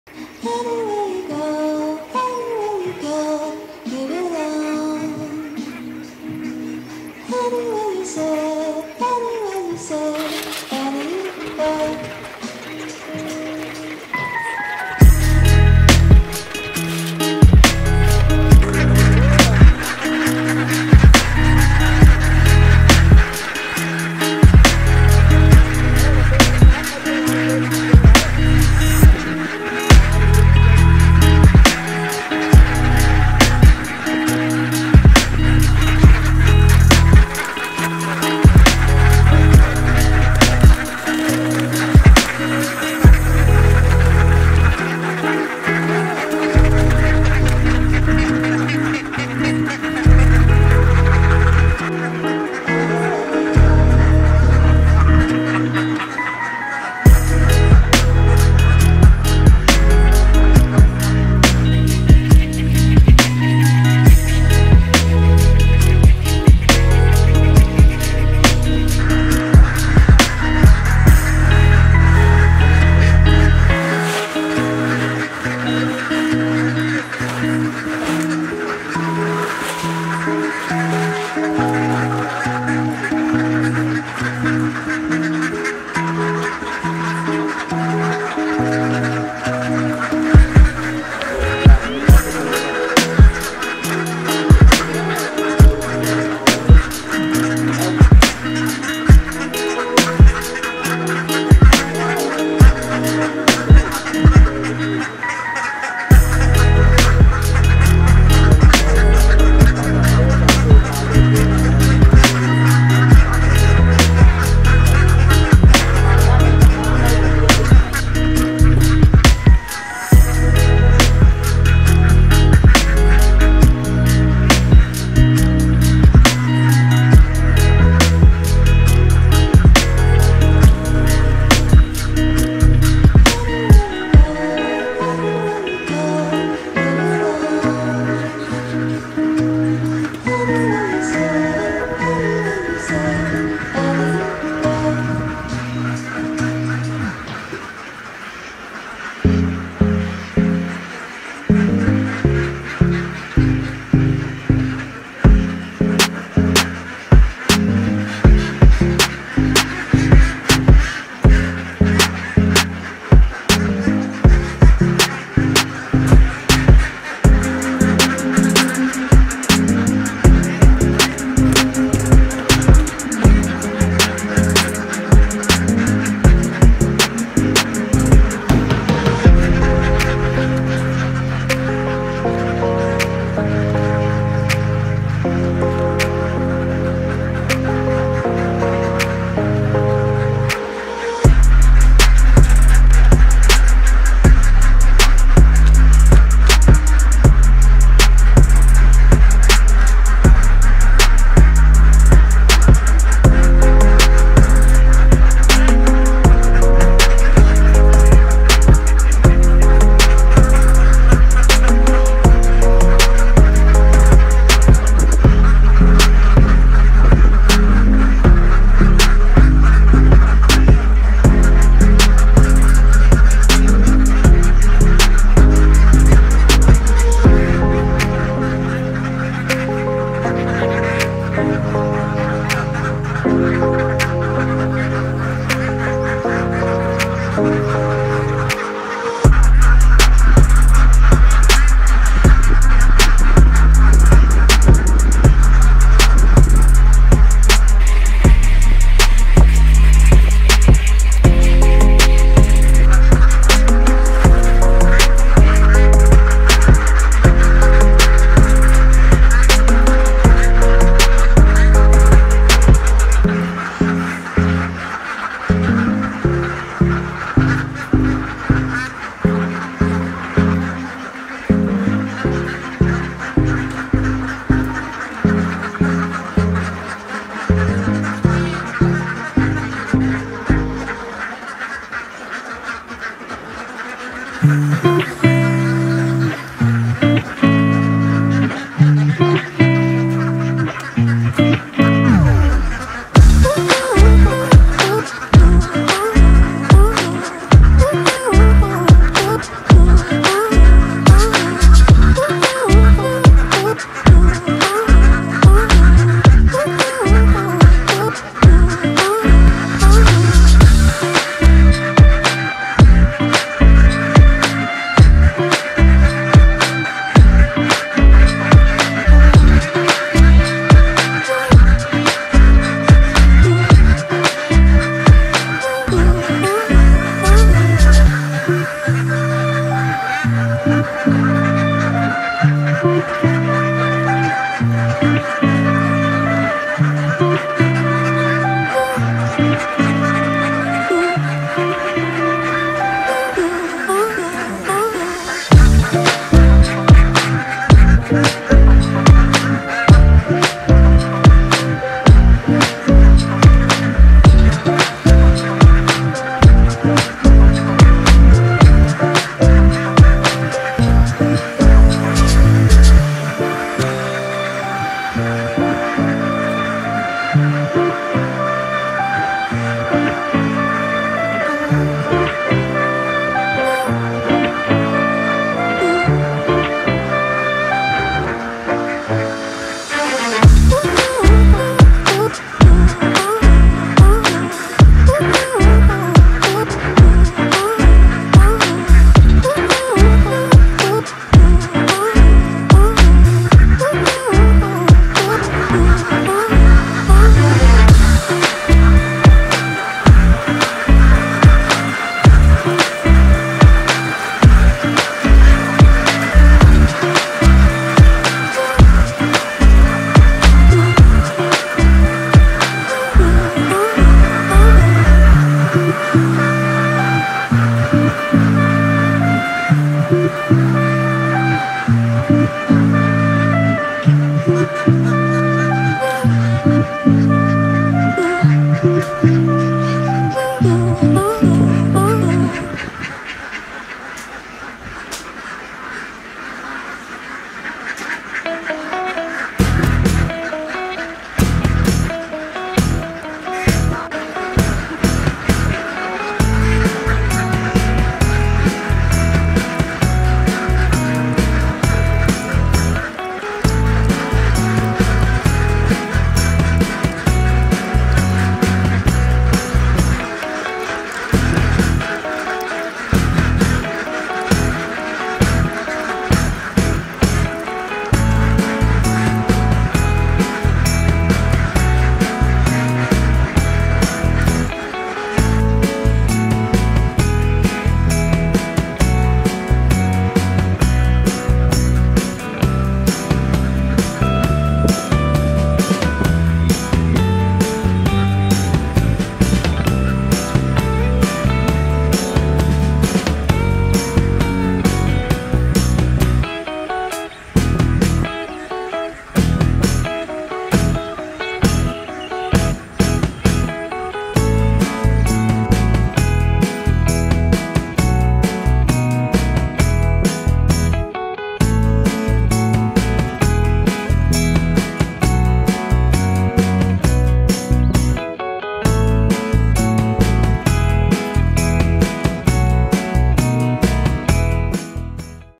Ducks Raising Method - Farm Raised 10 Thousand Ducks And Produce 10 Thousand Of Eggs Everyday
Hello guy, today I show you "Ducks Raising Method - Farm Raised 10 Thousand Ducks And Produce 10 Thousand Of Eggs Everyday".
It is agriculture technology in my country. It is important for farmer. You can follow me at your farm. Thank you so much.